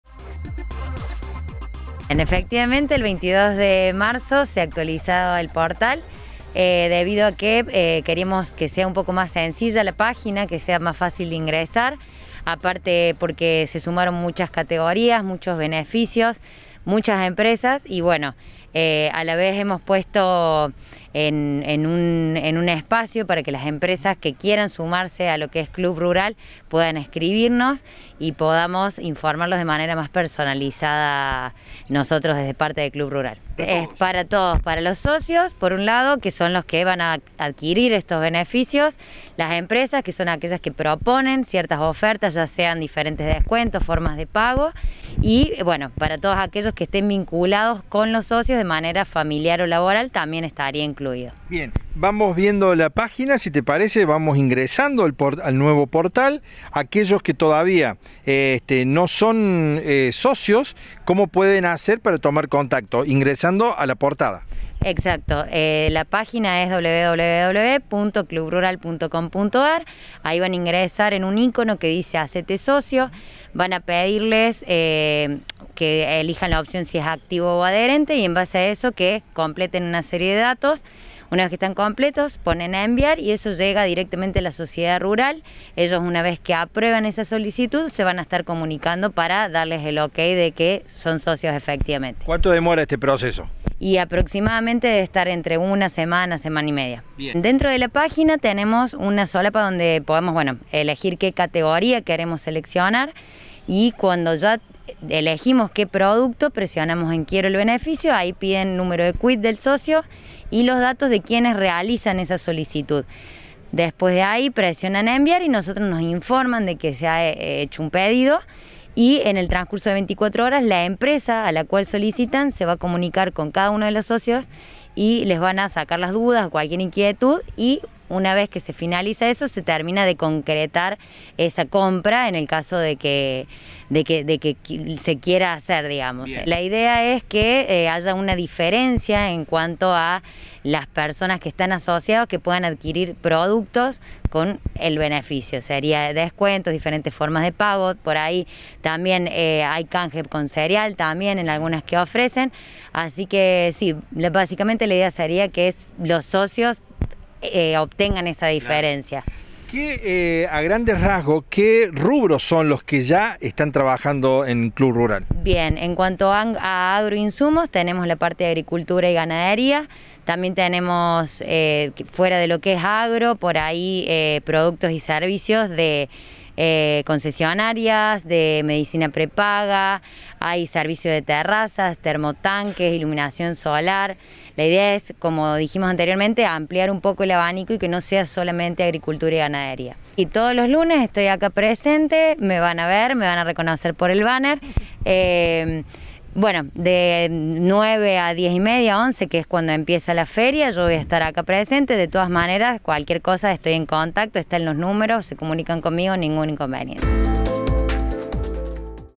Club Rural renueva su portal web
“Club Rural”, la innovadora propuesta de beneficios, renovó su portal de internet para agilizar las oportunidades de compra y venta de insumos para el agro.